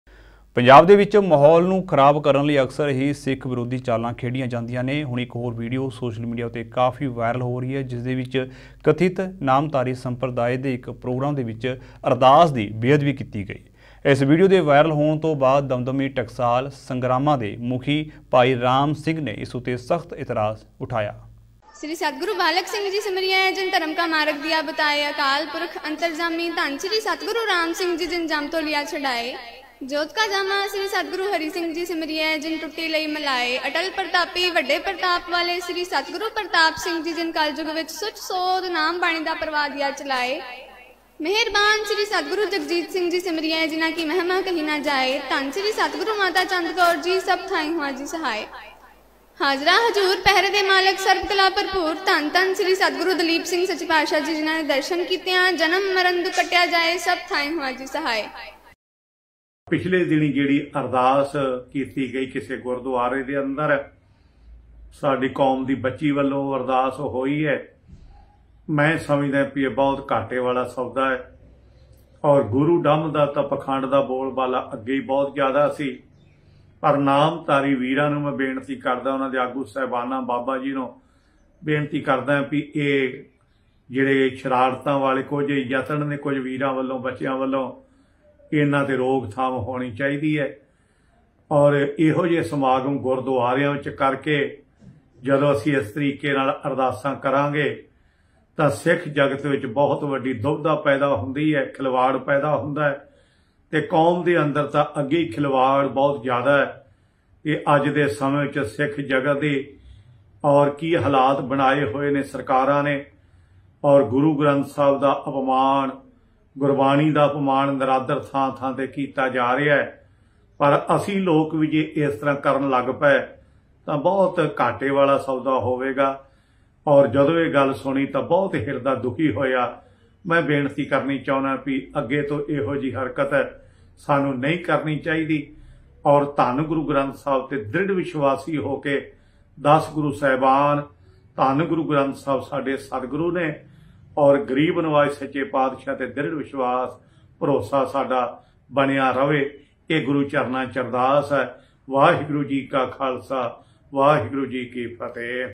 ਵਾਰ-ਵਾਰ ਕਿਉਂ ਸਿੱਖਾਂ ਦੇ ਧਾਰਮਿਕ ਜਜ਼ਬਾਤਾਂ ਨਾਲ ਖੇਡਿਆ ਜਾ ਰਿਹਾ ਹੈ ? ਵੇਖੋ ਕਿਵੇਂ ਅਰਦਾਸ ਦੀ ਕੀਤੀ ਗਈ ਬੇਅਦਬੀ !
ਵਾਰ-ਵਾਰ ਕਿਉਂ ਸਿੱਖਾਂ ਦੇ ਧਾਰਮਿਕ ਜਜ਼ਬਾਤਾਂ ਨਾਲ ਖੇਡਿਆ ਜਾ ਰਿਹਾ ਹੈ ? ਵੇਖੋ ਕਿਵੇਂ ਸਿੱਖਾਂ ਦੀ ਅਰਦਾਸ ਦੀ ਕੀਤੀ ਗਈ ਬੇਅਦਬੀ !